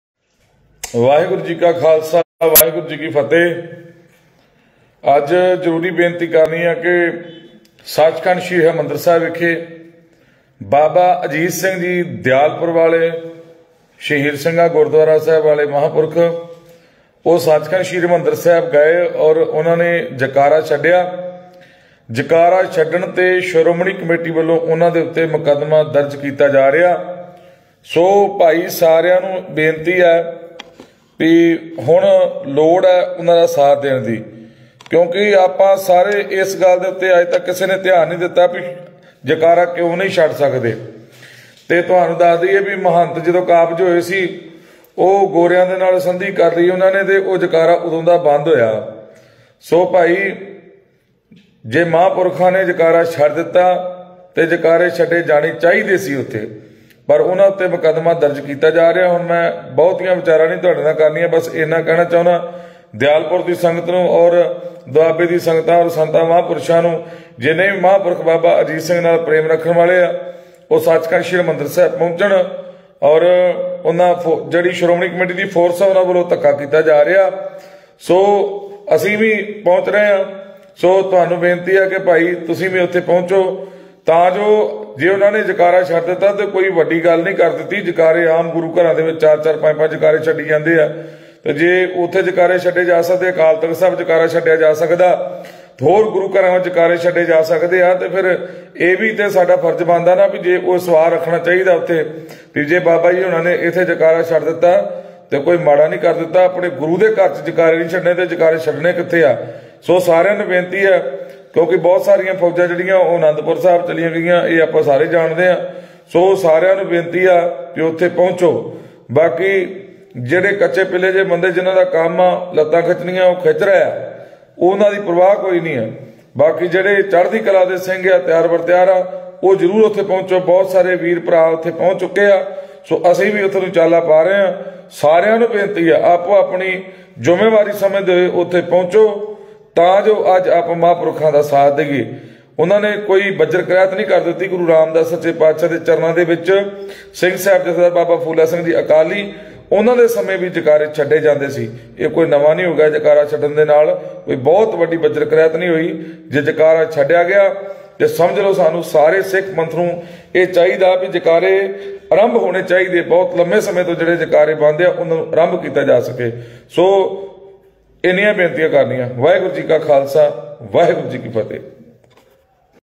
ਦਰਬਾਰ ਸਾਹਿਬ ਸੱਚ ਖੰਡ ਹਰਮਿੰਦਰ ਸਾਹਿਬ ਵਿਖੇ ਜੈਕਾਰੇ ਛੱਡਣ ਤੇ ਬਾਬਾ ਅਜੀਤ ਸਿੰਘ ਦਿਆਲਪੁਰ
ਦਰਬਾਰ ਸਾਹਿਬ ਸੱਚ ਖੰਡ ਹਰਮਿੰਦਰ ਸਾਹਿਬ ਵਿਖੇ ਜੈਕਾਰੇ ਛੱਡਣ ਤੇ ਬਾਬਾ ਅਜੀਤ ਸਿੰਘ ਦਿਆਲਪੁਰ ਵਲਿਆਂ ਤੇ ਕੀਤਾ ਜਾ ਰਿਹਾ ਹੈ ਮੁਕਦਮਾ ਸ਼੍ਰੋਮਣੀ ਕਮੇਟੀ ਵੱਲੋਂ ਸੰਗਤਾਂ ਨੂੰ ਬੇਨਤੀ ਹੈ ਪਹਾਉਚ ਕਰ ਕੇ ਬਾਬਾ ਜੀ ਦਾ ਸਾਥ ਦਿੱਤਾ ਜਾਵੇ